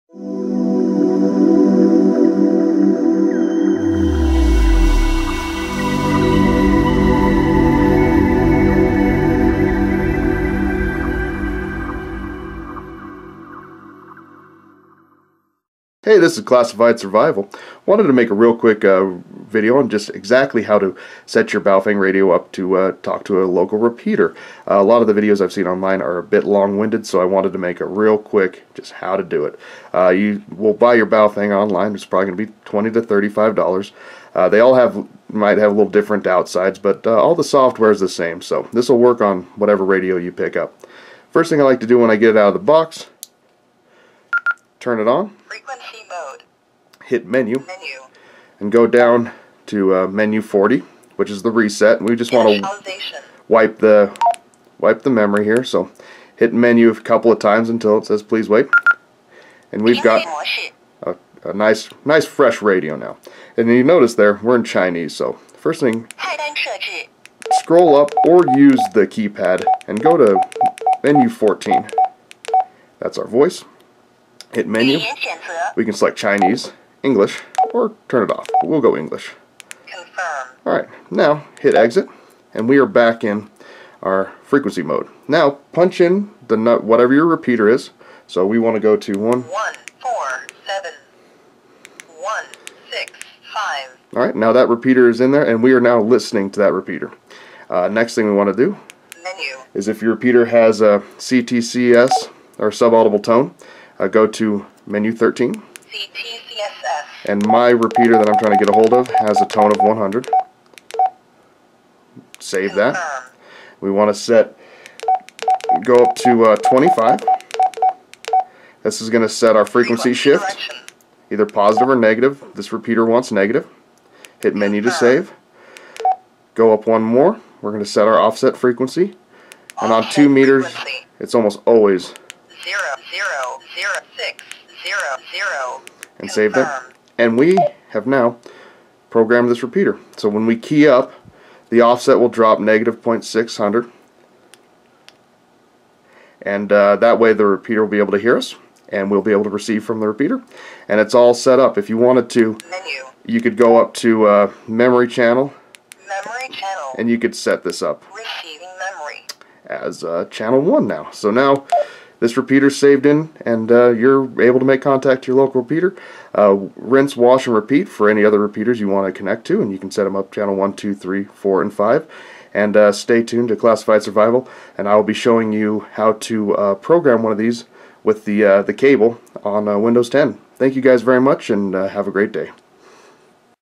Baofeng Radio Repeater Programing Quick&Easy
Here is a quick and easy guide to programming a repeater on your Baofeng radio.